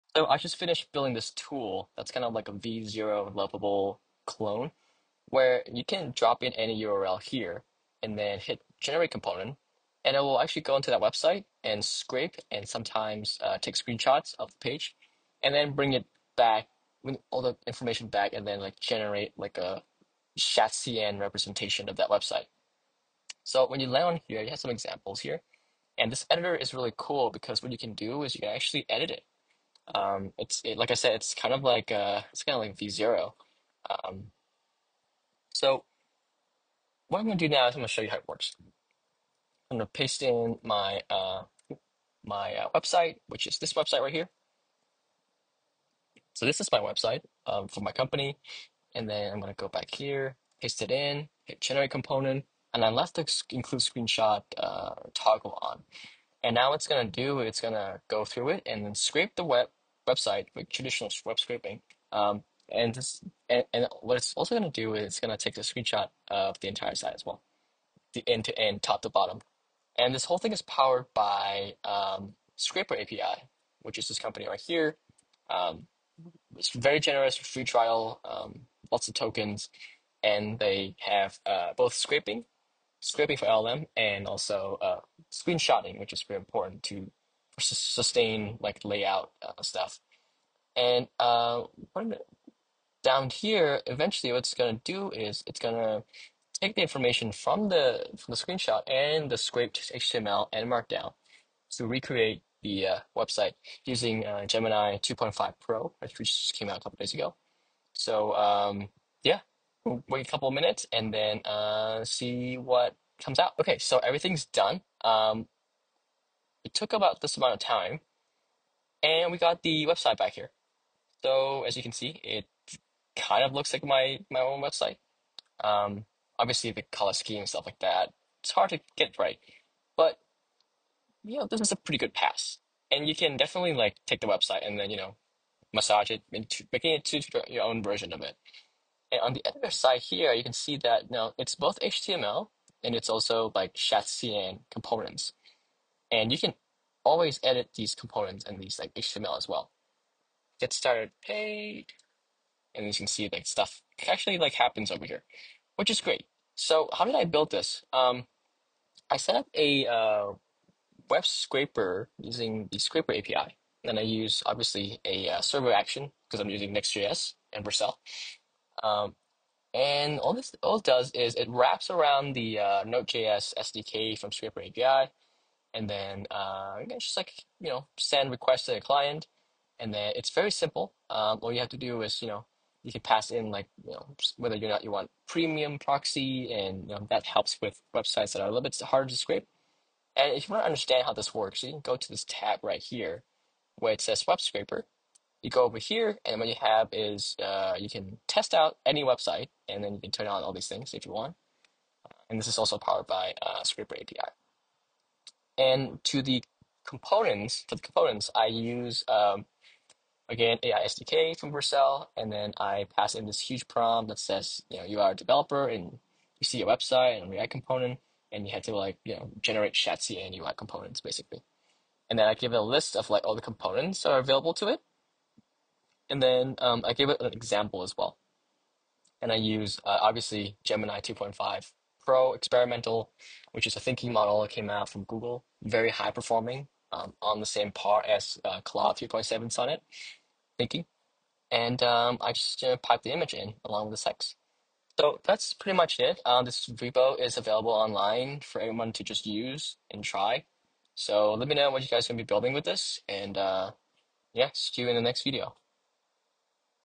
I built a tool (v0/Bolt.new inspired) to scrape + analyze any website (using URL)
Sign up for my upcoming AI Agents with OpenAI Agents SDK

Learn how to build a tool that scrapes any website so that you can analyze its structure. Then, use gemini-2.5-pro to generate a simple visualization and get inspired to create your own design.

Key points:
- Scraping a website with ScraperAPI (you can use screenshot-to-code)
- Generating a design inspired by your target site using gemini-2.5-pro
- Editing and customizing visualized components